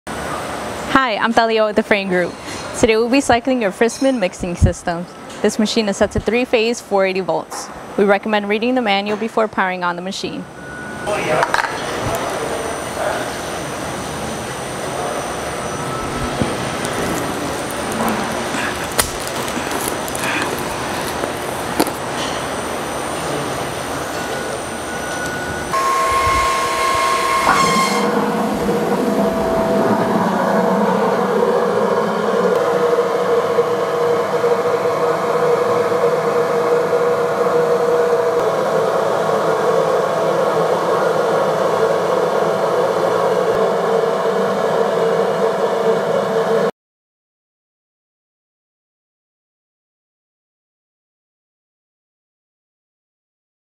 Fristam Pumps 20-53 Liquid Mixer Demonstration
This is the demonstration of the Fristam Pumps 20-53 Liquid Mixer.

Fristam Model 20-53, 316 stainless steel, in-line, integrated mixing system, incorporates Model FZX-2200 liquid ring pump with Model FS-3532 high shear mixer / emulsifier. Equipped with 23-1/2" OD x 24" long stainless steel conical dry product hopper with safety grid and lift off cover. Fristam Model FZX-2200 liquid ring pump has 10" OD / 8" ID front plate with 8" OD impeller and front 2-1/2" OD liquid product in feed port with 15 Hp motor drive. Model FS-3532 high shear mixer has 7" OD rotor / stator mixing head with 25 Hp motor drive, both and sanitary 2-1/2" OD sanitary ports. Control panel has FZX / FS pump and blender start, stop, e-stop push buttons, main machine / vibrator on / off switches. Mounted on (4) leg stainless steel frame. Rated from 55 to 120 liquid gallons and 23 to 225 pounds of dry product per minute - depending on materials and application